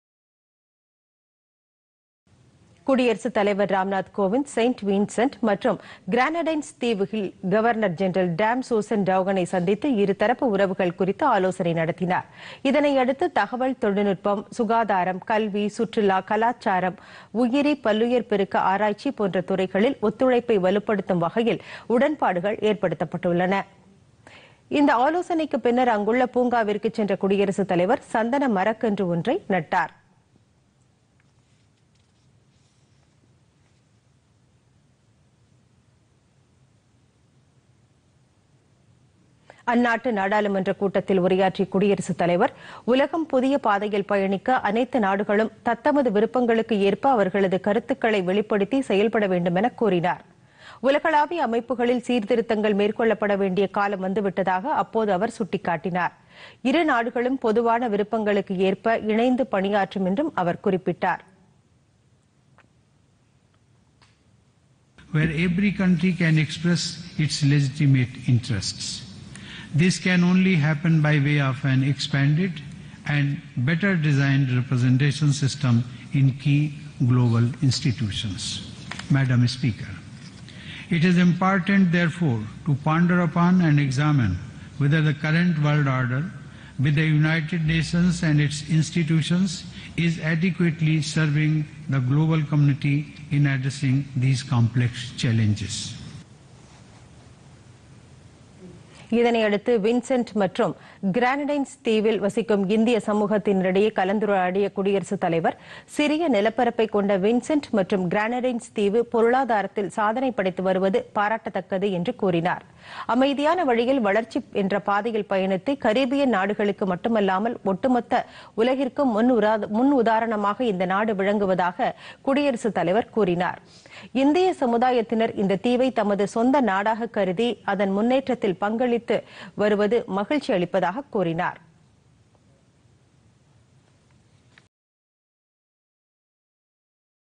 வளர்ச்சி அனுபவத்தை பகிர்ந்து வரும் இந்தியாPodhigaiTamilNews
வளர்ச்சிப் பயணத்தில் பெற்ற அனுபவத்தை சக வளரும் நாடுகளுடன் பகிர்ந்த கொள்வதில் இந்தியா உறுதியுடன் உள்ளது - குடியரசுத் தலைவர் ராம்நாத் கோவிந்த்.